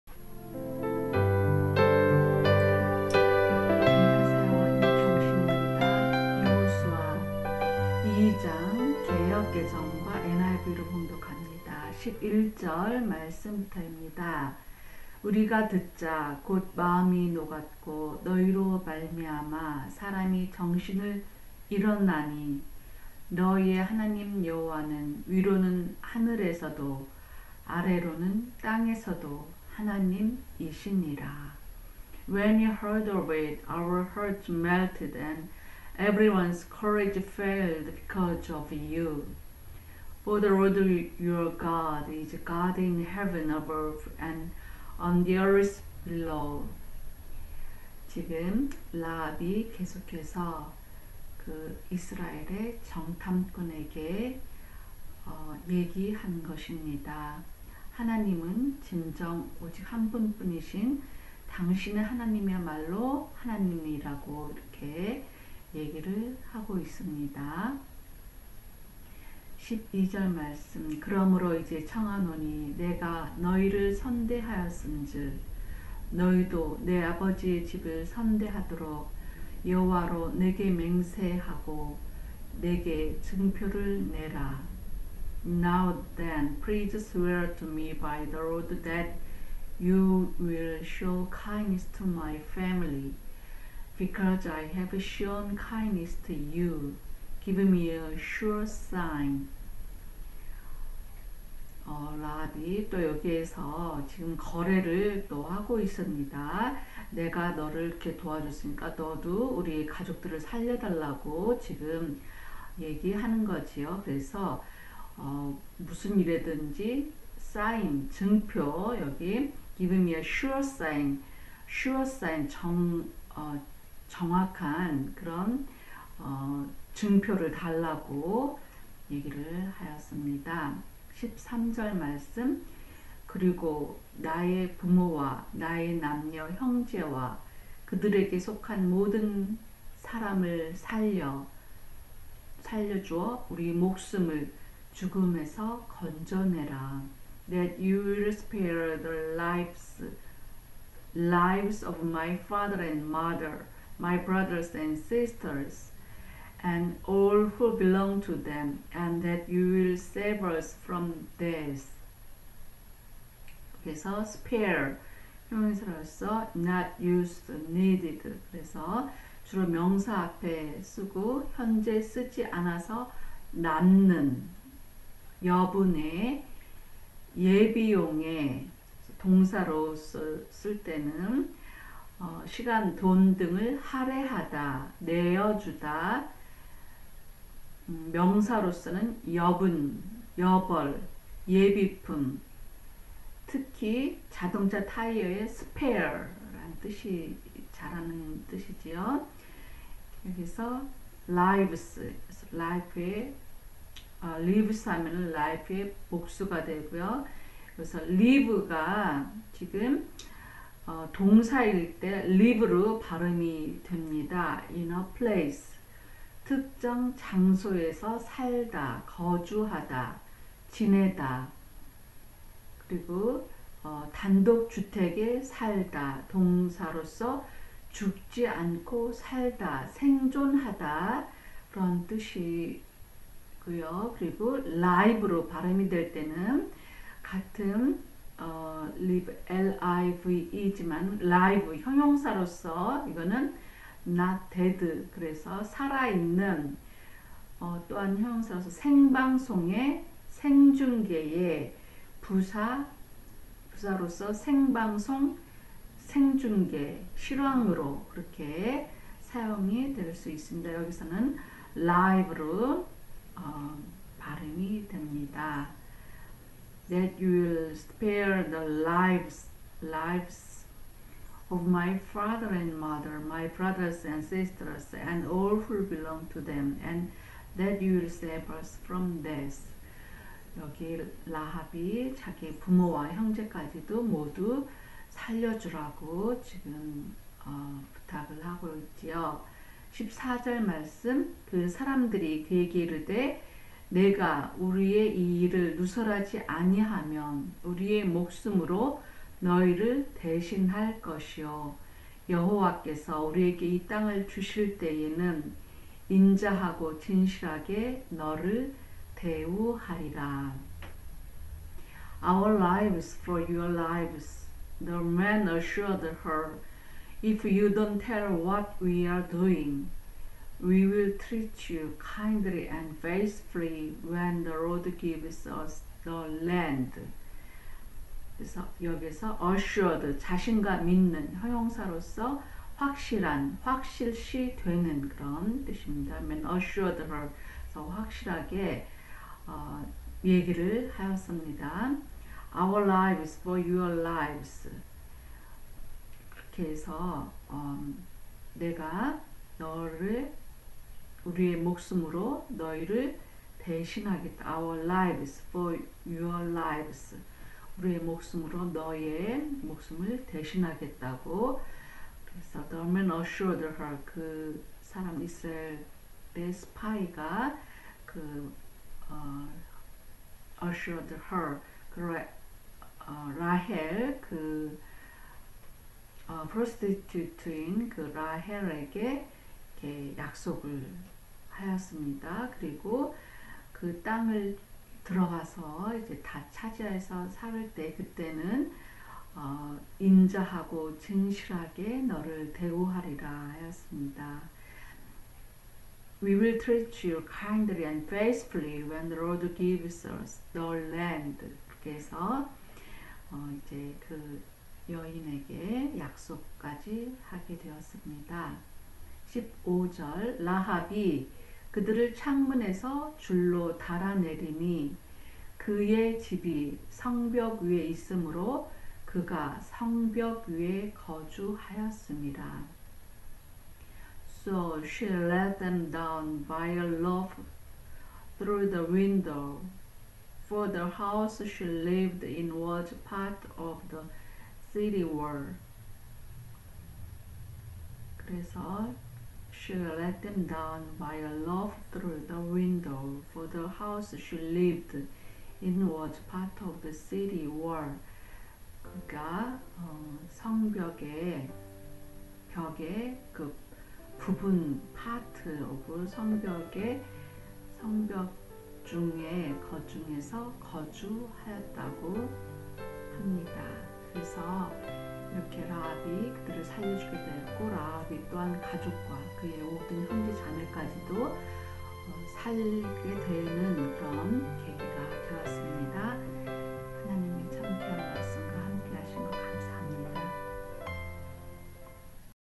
366 Joshua 2:11~15 여호수아 Mpeg-2 Hi QT Sharing by Kyong S Lee Mary 이 경신 유럽 선교사와 함께
여호수아 2장 11~15 절 말씀 [개역개정:NIV]

여호수아 2장 [개역개정:NIV]

11.  우리가 듣자 곧 마음이 녹았고 너희로 말미암아 사람이 정신을 잃었나니 너희의 하나님 여호와는 위로는 하늘에서도 아래로는 땅에서도 하나님이시니라
When we heard of it, our hearts melted and everyone's courage failed because of you, for the LORD your God is God in heaven above and on the earth below.

12.  그러므로 이제 청하노니 내가 너희를 선대하였은즉 너희도 내 아버지의 집을 선대하도록 여호와로 내게 맹세하고 내게 증표를 내라
Now then, please swear to me by the LORD that you will show kindness to my family, because I have shown kindness to you. Give me a sure sign

13.  그리고 나의 부모와 나의 남녀 형제와 그들에게 속한 모든 사람을 살려 주어 우리 목숨을 죽음에서 건져내라
that you will spare the 라이브스 lives of my father and mother, my brothers and sisters, and all who belong to them, and that you will save us from death."
spare [sper]  [형용사] NOT USED/NEEDED | (주로 명사 앞에 씀) (현재 쓰지 않아서) 남는,
여분의, 예비용의, [동사] (시간・돈 등을) 할애하다 [내다 / 내어 주다] , [명사] 여분, 여벌, 예비품, (특히 자동차 타이어) 스페어
lives: LIFE 의 복수
live  [lɪv]  [동사] IN A PLACE | (특정 장소에(서)) 살다 [거주하다/지내다], (단독) 주택에 살다,  [동사] (죽지 않고) 살다 [생존하다]
live  [laɪv]  [형용사] NOT DEAD | 살아 있는,  [형용사] 생방송의, 생중계의, [부사] 생방송 [생중계/실황] 으로

14.  그 사람들이 그에게 이르되 네가 우리의 이 일을 누설하지 아니하면 우리의 목숨으로 너희를 대신할 것이요 여호와께서 우리에게 이 땅을 주실 때에는 인자하고 진실하게 너를 대우하리라
"Our 라이브스 lives for your 라이브스 lives!" the men assured her. "If you don't tell what we are doing, we will treat you kindly and faithfully when the LORD gives us the land."
assured [ə|ʃʊrd]   자신감 있는,  [형용사] 확실한, 확실시 되는

15.  라합이 그들을 창문에서 줄로 달아 내리니 그의 집이 성벽 위에 있으므로 그가 성벽 위에 거주하였음이라
So she let them down by a rope through the window, for the house she lived in was part of the city wall.

종합 법률 / 이민 업무 전문
PROFESSIONAL LEGAL SERVICE

12345 KENTUCKY ST.